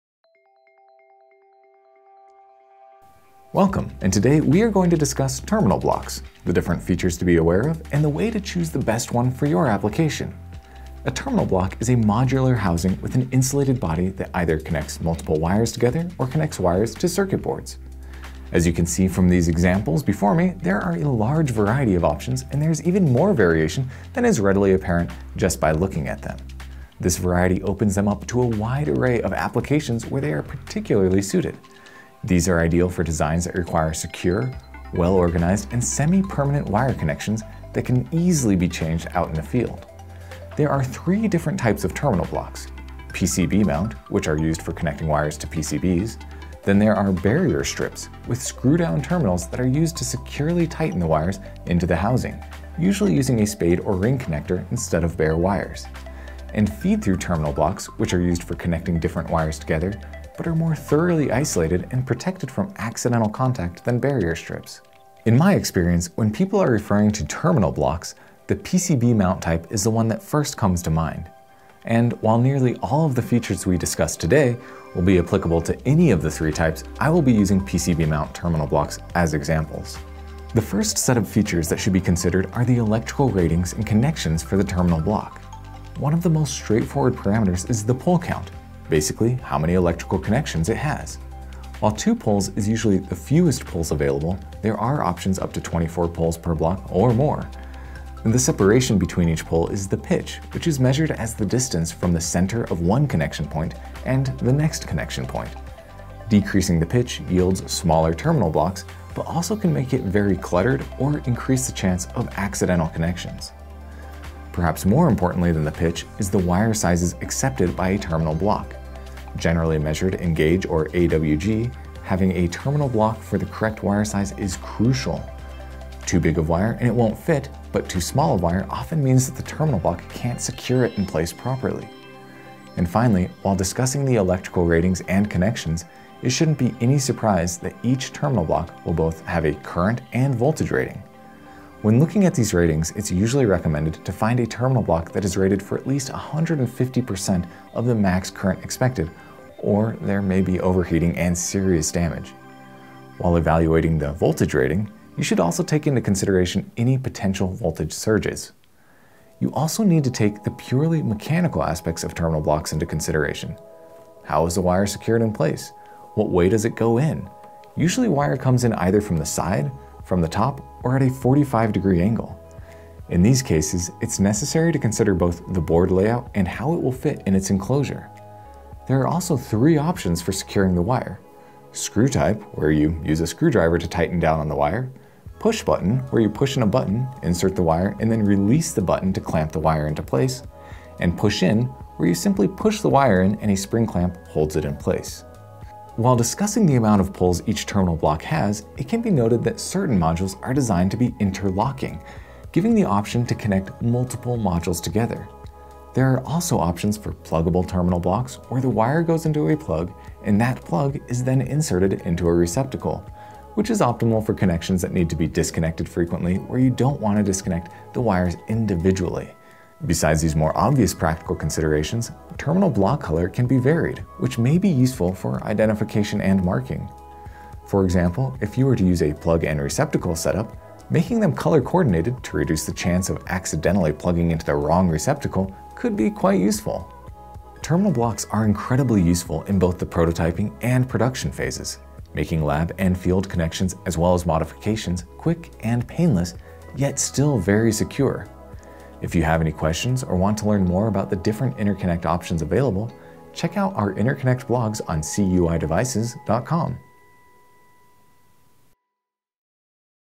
The Ultimate Guide to Terminal Block Selection
Terminal blocks are used in a wide array of applications requiring semi-permanent wire connections that can be easily swapped out for inspection or repair in the field. While simple in nature, there are a variety of options and specifications to consider when choosing a terminal block for your design. This video serves as a comprehensive resource for terminal block selection, discussing types and configurations, key electrical and mechanical considerations, and more.

Read more in our The Ultimate Guide to Terminal Block Selection blog

0:00 Introduction
0:11 What is a Terminal Block?
0:40 Types of Terminal Blocks
1:20 Key Terminal Block Electrical Considerations
2:39 Key Terminal Block Mechanical Considerations
3:57 Terminal Block Uses